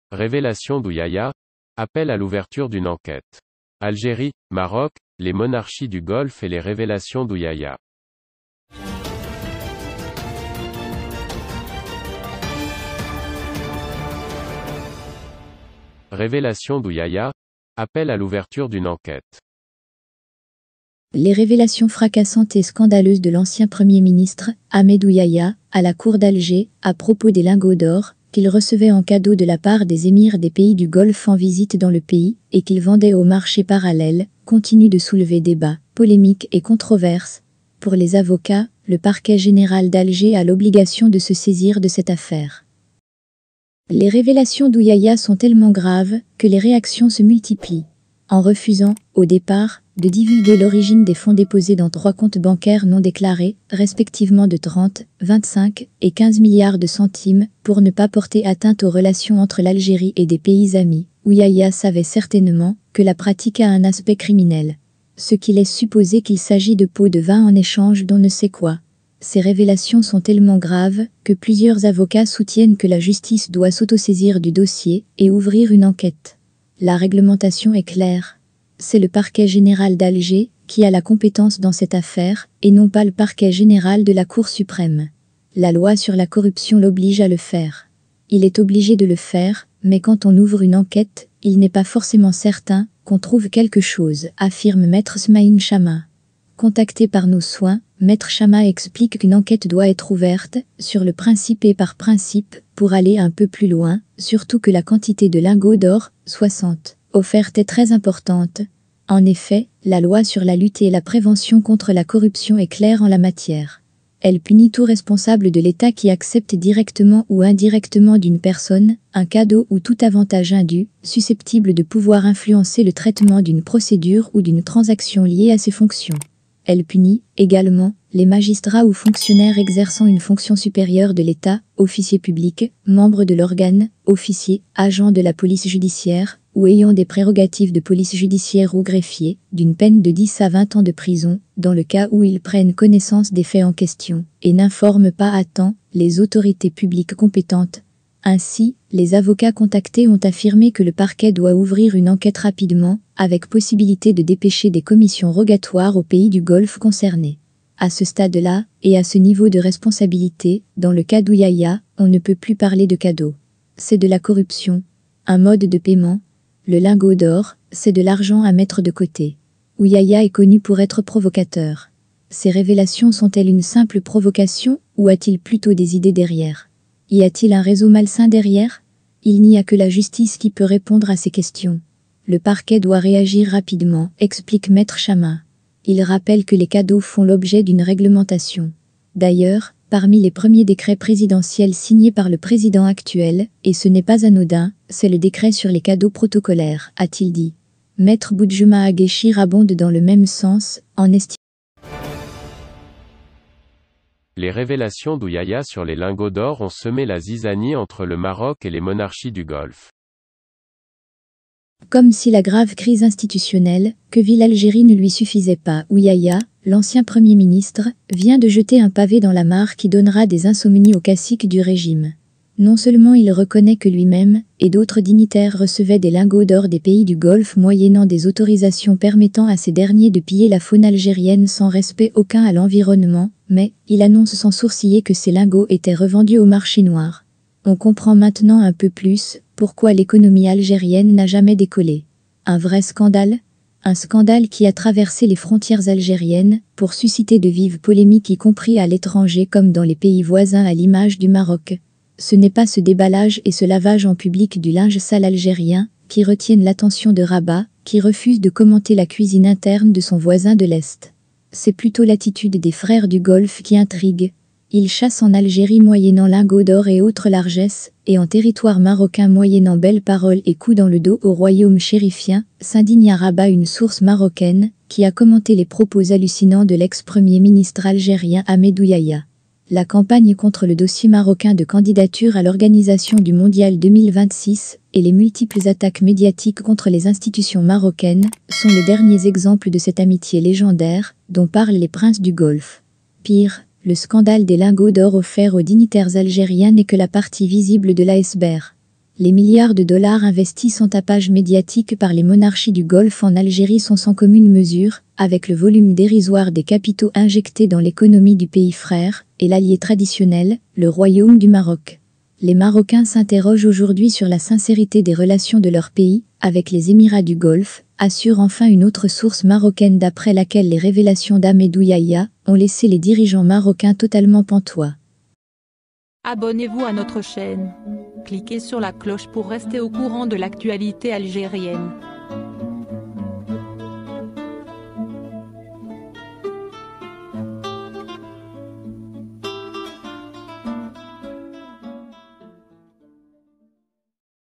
Algérie, Maroc, les émirs du Golfe et les Révélations d’Ouyahia | Ouyahia : Ouverture d’une enquête
Les révélations fracassantes et scandaleuses de l’ancien Premier ministre, Ahmed Ouyahia, à la cour d’Alger, à propos des lingots d’or qu’il recevait en cadeau de la part des émirs des pays du Golfe en visite dans le pays et qu’il vendait au marché parallèle, continuent de soulever débat, polémique et controverse. Pour les avocats, le parquet général d’Alger a l’obligation de se saisir de cette affaire.

Comme si la grave crise institutionnelle que vit l’Algérie ne lui suffisait pas, Ouyahia, l’ancien premier ministre, vient de jeter un pavé dans la mare qui donnera des insomnies aux caciques du régime. Non seulement il reconnaît que lui même et d’autres dignitaires recevaient des lingots d’or des pays du Golfe moyennant des autorisations permettant à ces derniers de piller la faune algérienne sans respect aucun à l’environnement, mais il annonce sans sourciller que ces lingots étaient revendus au marché noir.